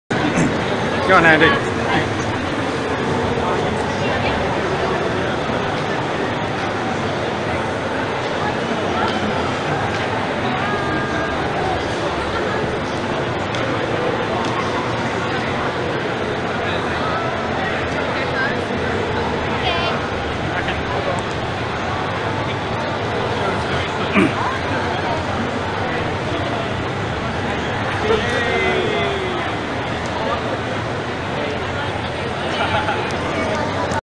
Andy meets Charisma.com
Andy meeting Itsuka and Gonchi from Charisma.com at Hyper Japan London at 16 July 2016